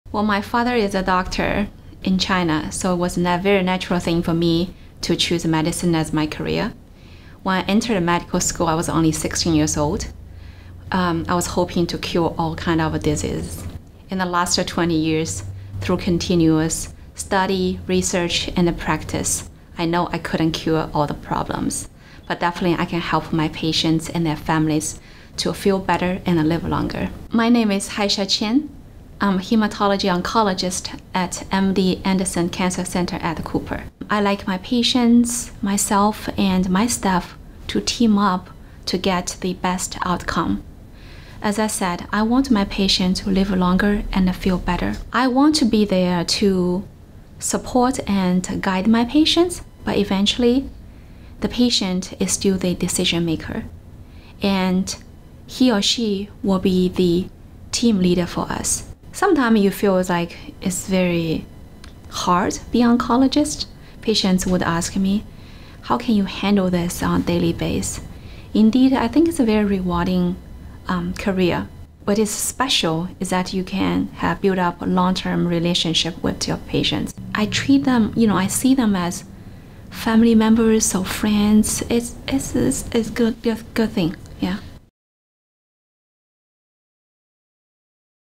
Meet Dr. Haixia Qin | Hematologist/Medical Oncologist | MD Anderson Cancer Center at Cooper
Meet Dr. Haixia Qin, a Hematologist/Medical Oncologist at MD Anderson Cancer Center at Cooper.

To learn more about our comprehensive cancer services and our team approach to patient care, visit: MDAndersonCooper.org.